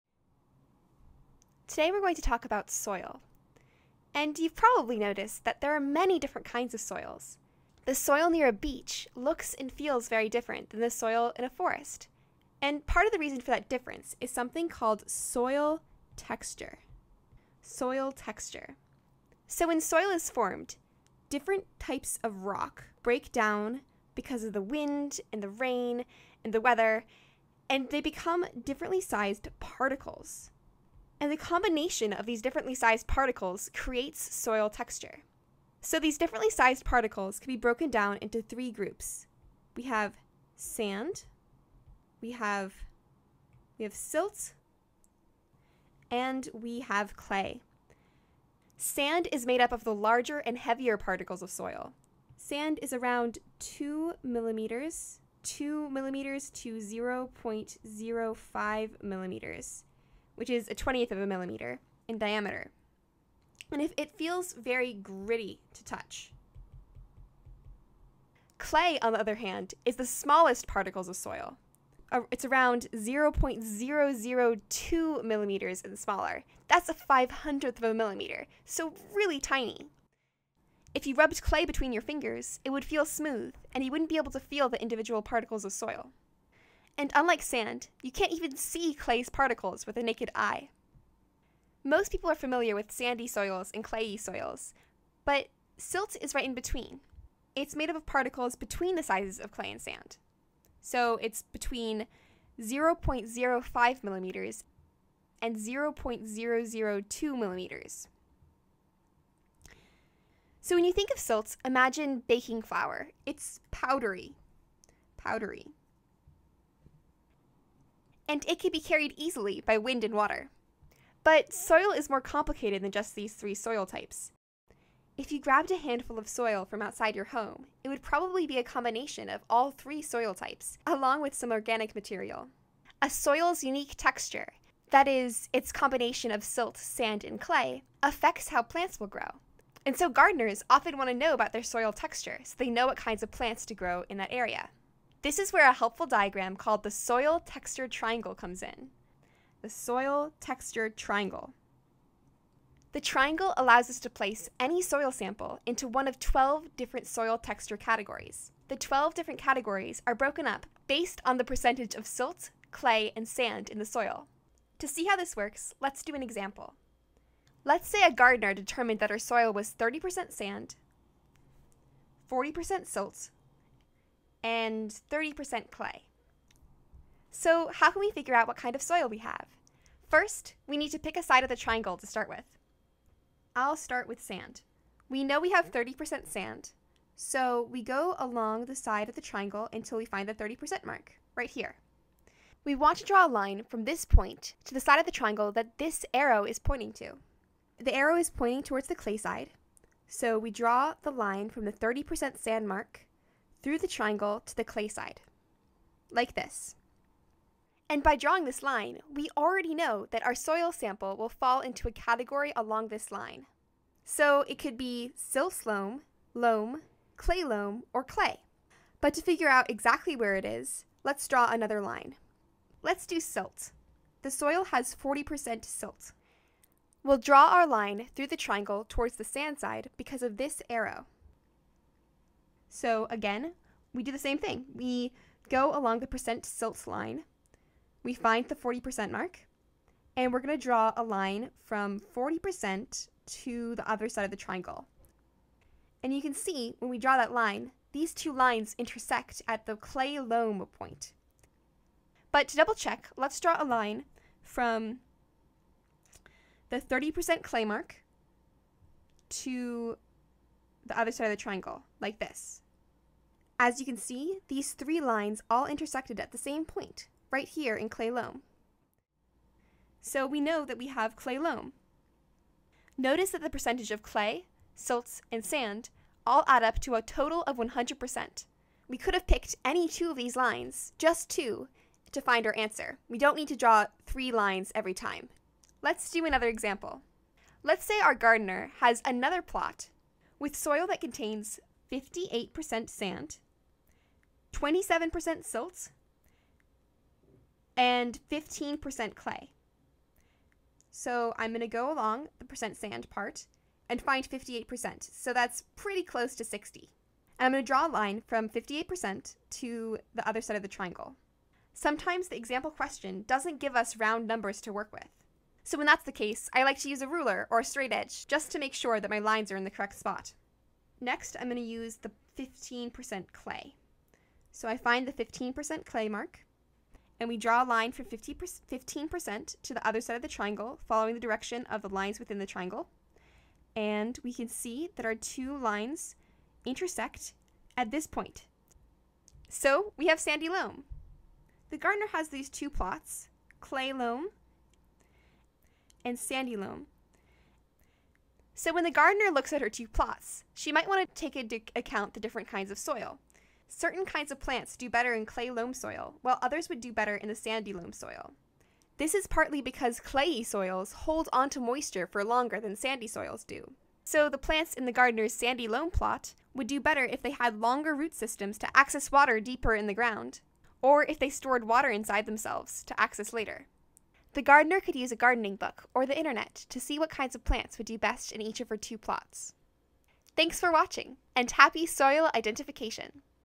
Soil Texture Triangle| Earth systems and resources| AP environmental science| Khan Academy
Practice identifying and comparing soil types based on their percentage of clay, silt, and sand.